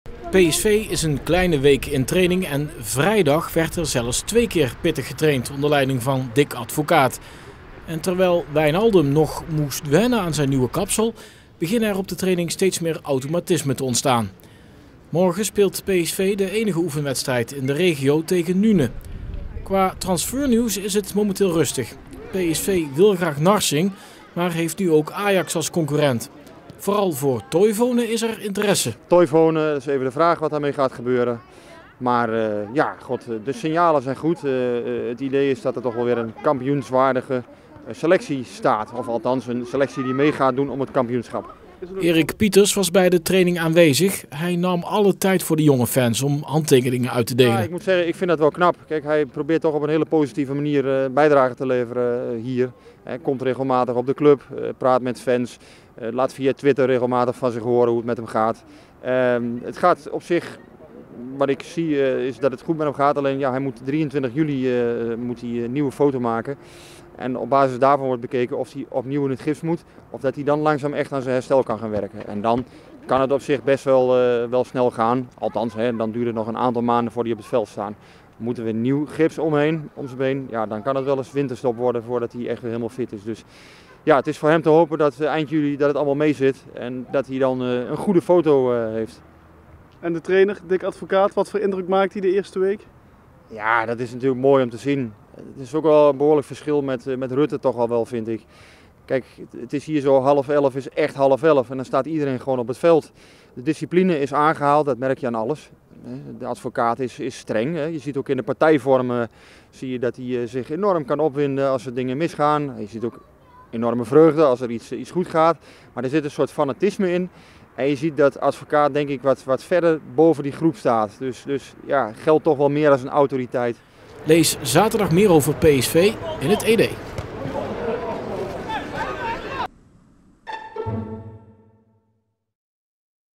PSV blijft bezig met Narsingh
PSV houdt er nog altijd rekening mee dat Luciano Narsingh voor PSV gaat kiezen. De aanvaller van Heerenveen maakt vermoedelijk komende week bekend waar hij dit seizoen gaat spelen: bij PSV, of Ajax.